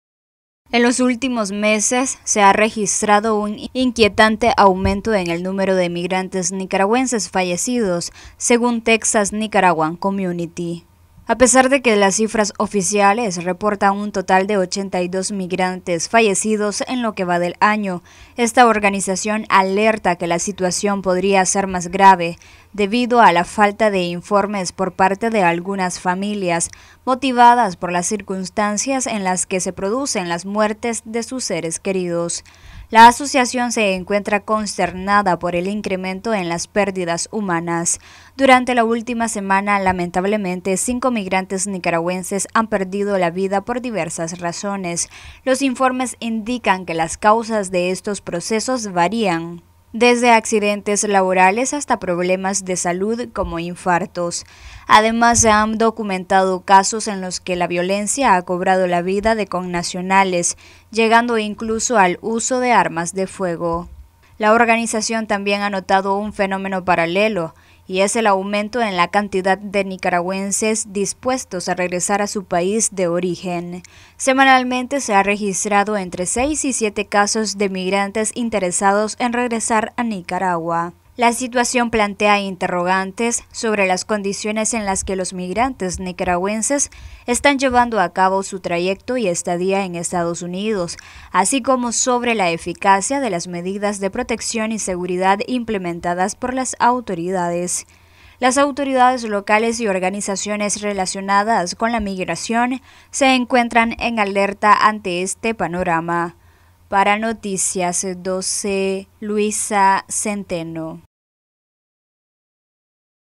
Noticias12Nicaragua Aumentan cifras de migrantes fallecidos en EE.UU
Aumentan cifras de migrantes fallecidos en EE.UU y connacionales dispuestos a regresar